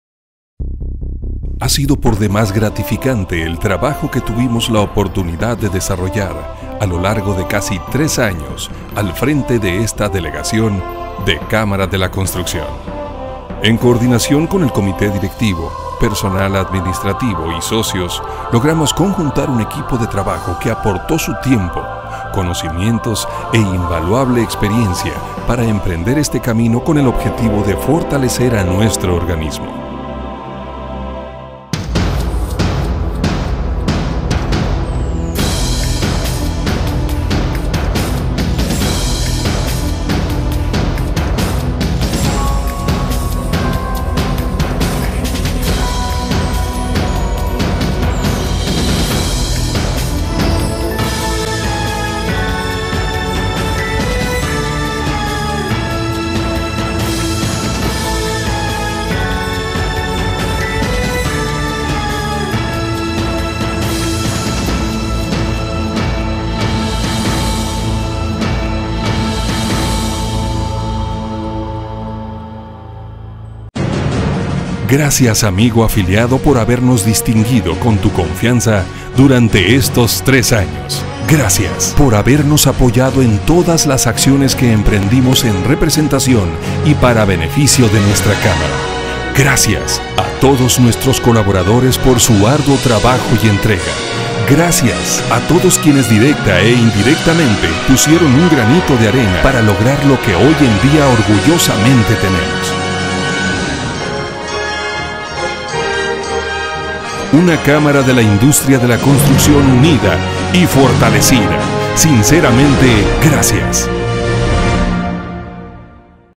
CMIC Inforrme
Informe de Actividades de la Cámara Mexicana de la Industria de la Construcción - Delegación Baja California. . . . . Incluye entrada y salida del documental . . .  producido por ADeO Producciones